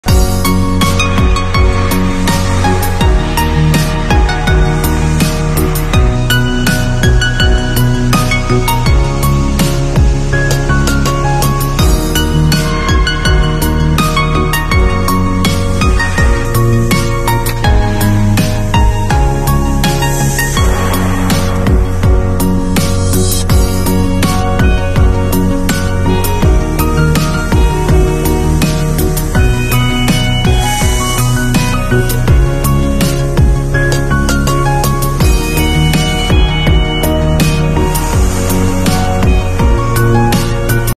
Accuracy Test
The technician is measuring the thickness of the steel with an instrument.